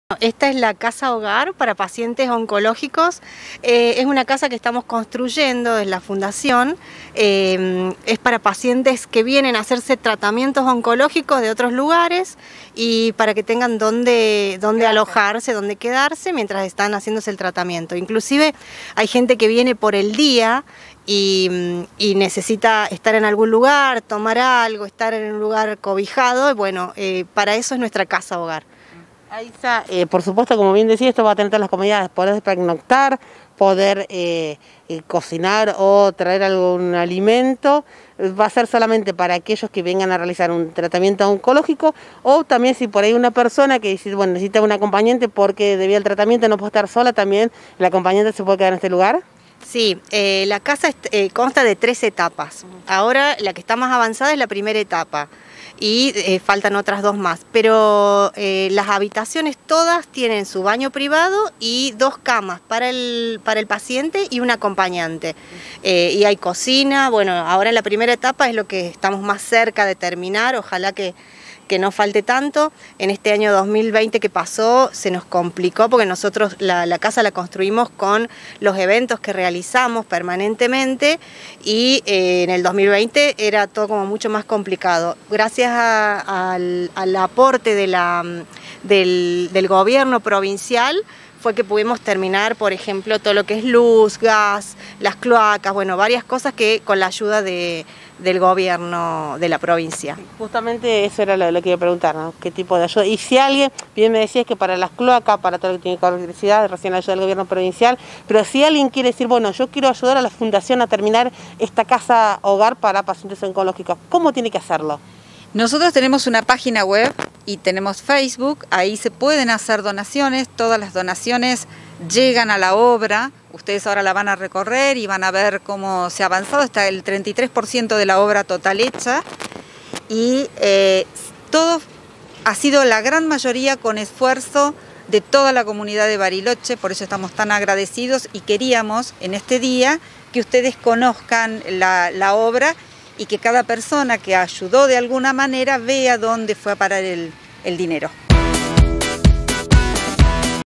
Aixa Terrera y Susana Ferrari - fundación cerca tuyo -
Están construyendo una casa hogar para pacientes oncologicos